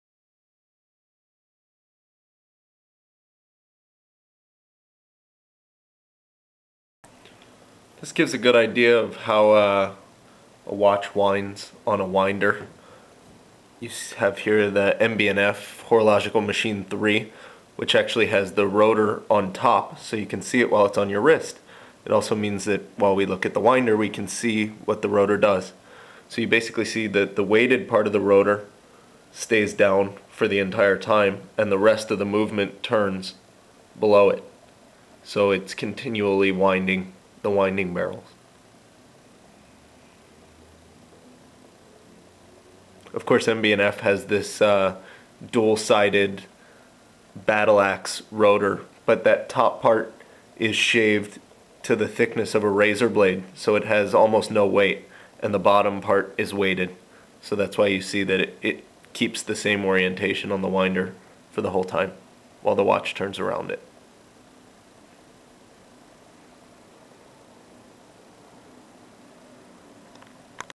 How a Watch Winder Works
Stephen Hallock demonstrates how a watch winder actually winds an automatic watch using an MB&F Horological Machine #3 (HM3), a watch with the automatic rotor visible from the top